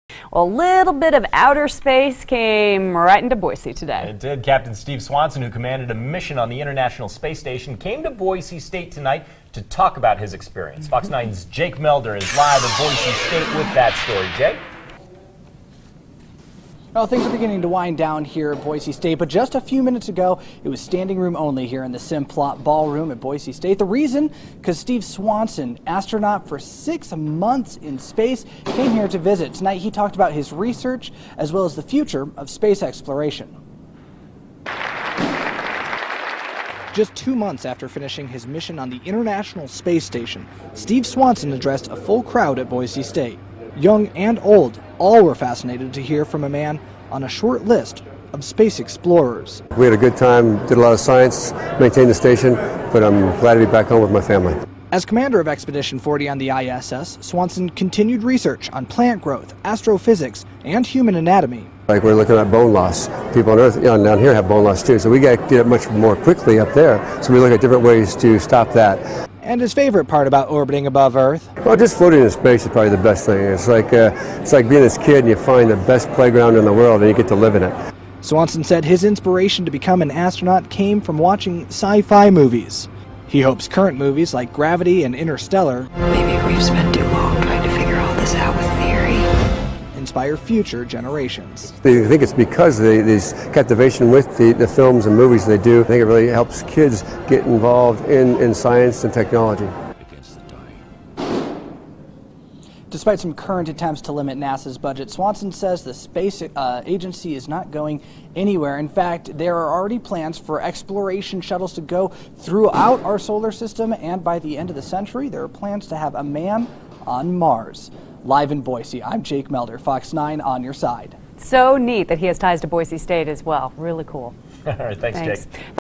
"Space Bronco" returns to Idaho
Astronaut Steve Swanson just returned from International Space Station